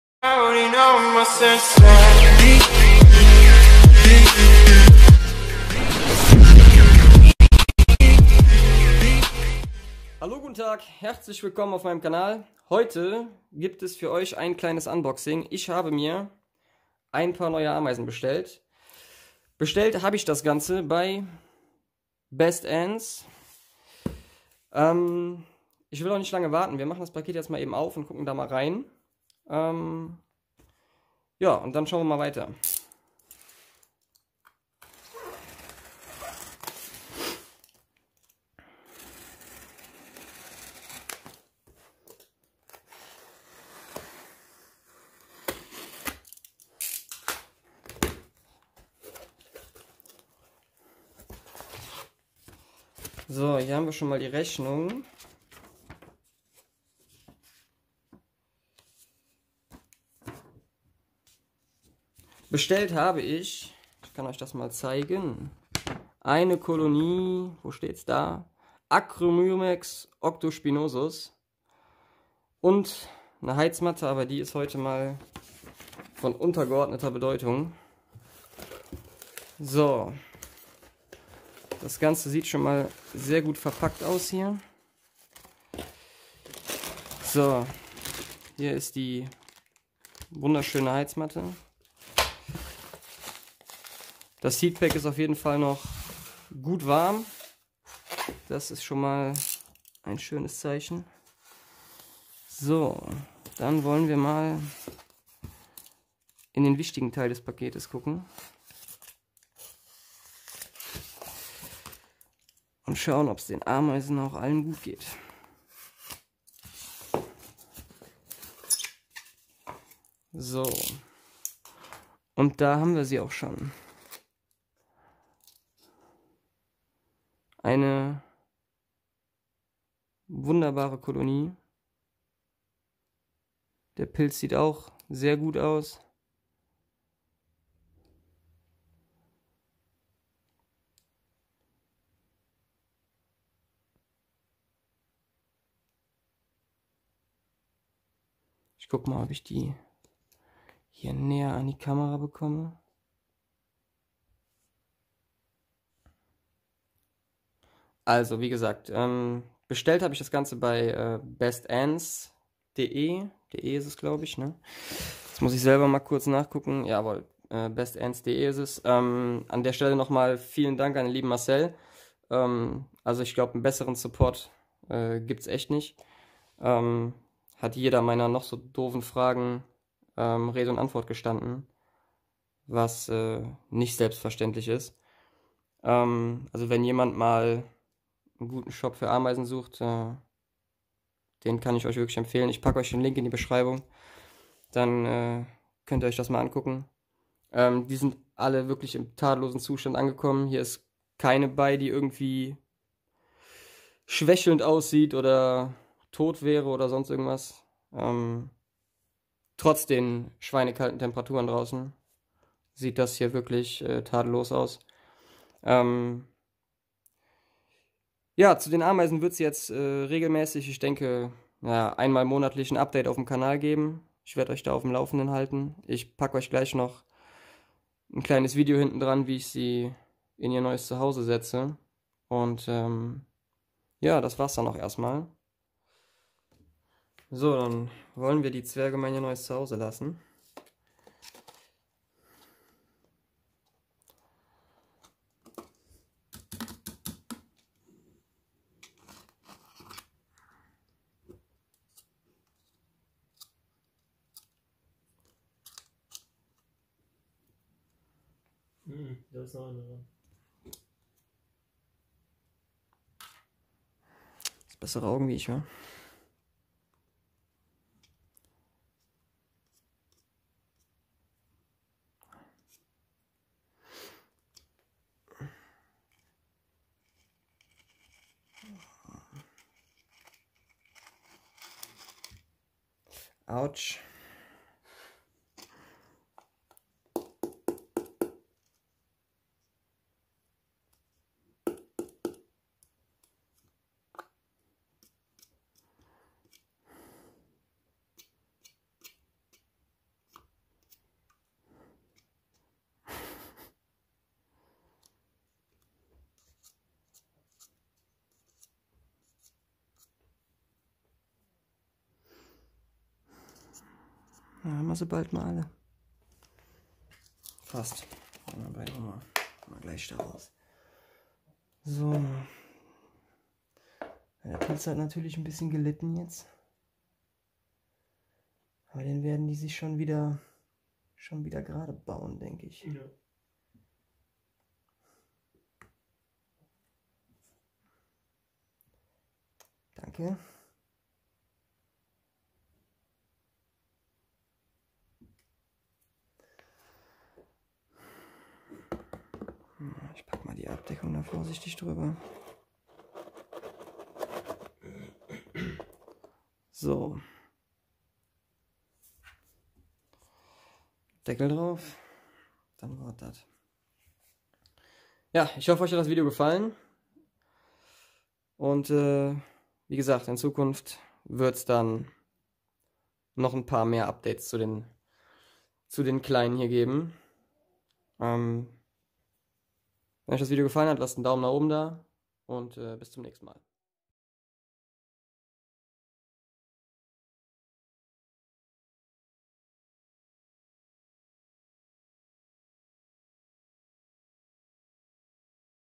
Acromyrmex octospinosus Unboxing! Blattschneiderameisen! Terraristik Ultras!
Unboxingvideo meiner neuen Acromyrmex octospinosus Kolonie!